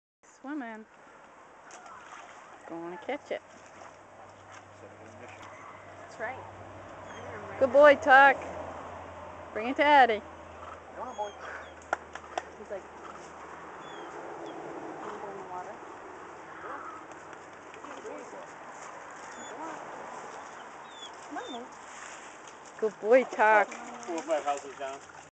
Tucker Whitney
Our Boykin Spaniel Tucker swimming at Silver Lake SP in Hollis NH